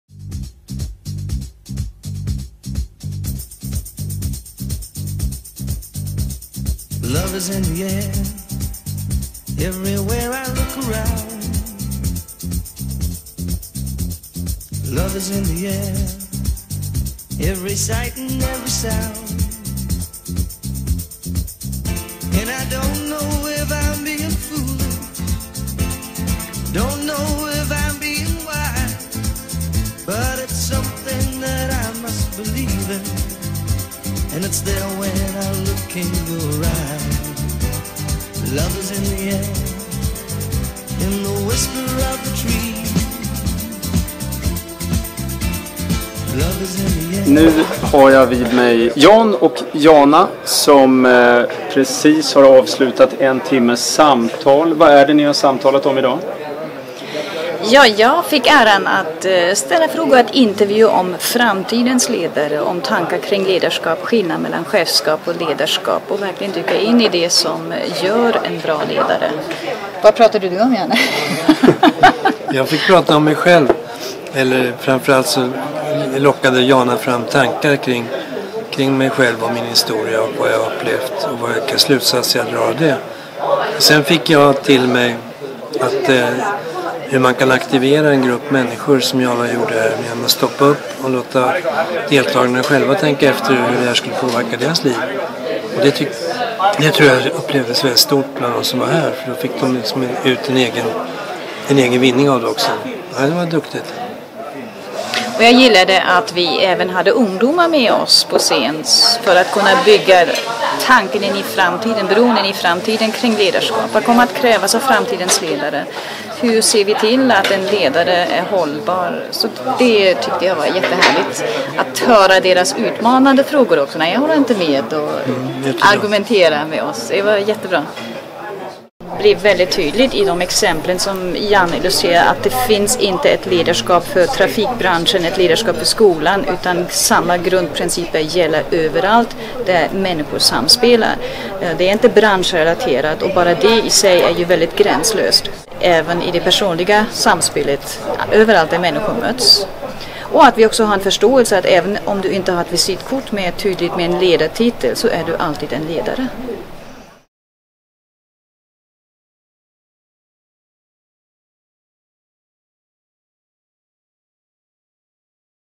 Riv Pyramiderna med Jan Carlzon
Ett unikt, annorlunda och gränslöst samtal mellan Jana Söderberg och den legendariska företagsledaren Jan Carlzon.